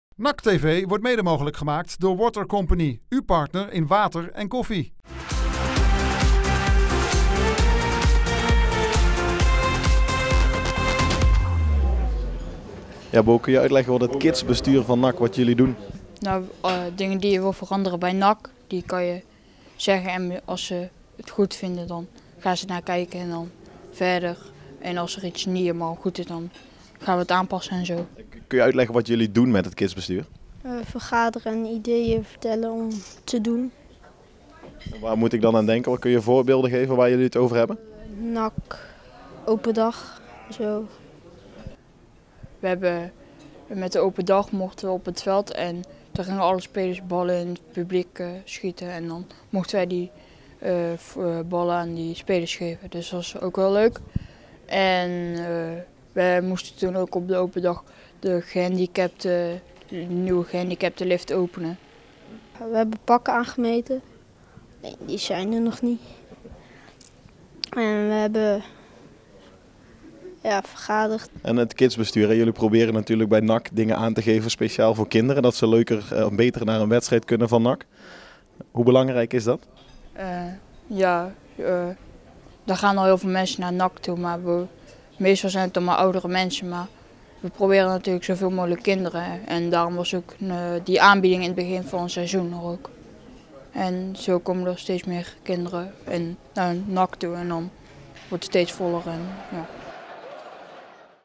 NAC TV 19/11: Het Kidsbestuur van NAC
Vandaag in NAC TV: de leden van het Kidsbestuur leggen uit wat hun taak is bij de club.